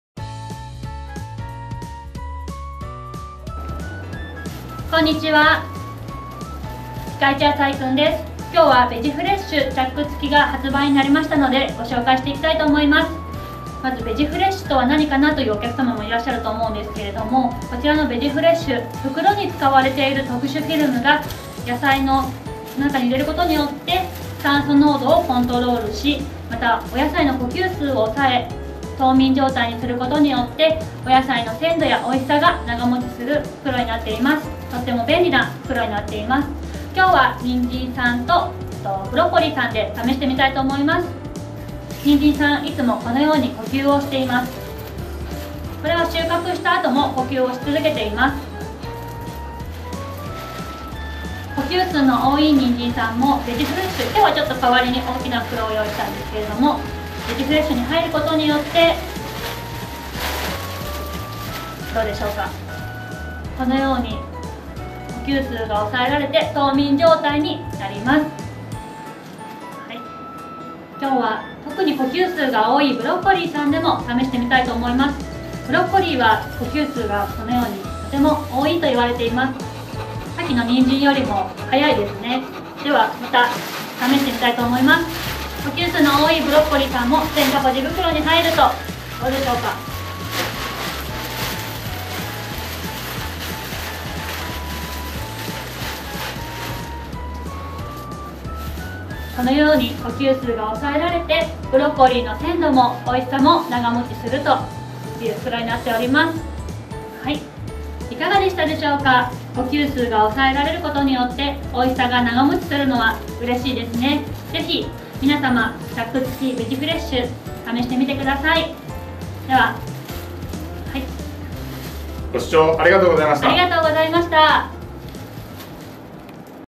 【人参の呼吸】野菜長持ち！鮮度保持袋ベジフレッシュのご紹介【鮮度保持袋】
もっと詳しく知りたい方はLINEでお気軽にお問合せください。
フリーダイヤルはこちら→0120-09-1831

こんにちは！
ピカイチ野菜くんです。

今回は、鮮度保持袋ベジフレッシュをピカイチ野菜くんスタッフがわかりやすくご紹介します！

ベジフレッシュは、野菜の呼吸を抑えることによって、鮮度や美味しさを保ってくれる鮮度保持袋です。
ピカイチ野菜くんスタッフが野菜になりきって説明します！

※安全のため真似をして袋に入る事はおやめください※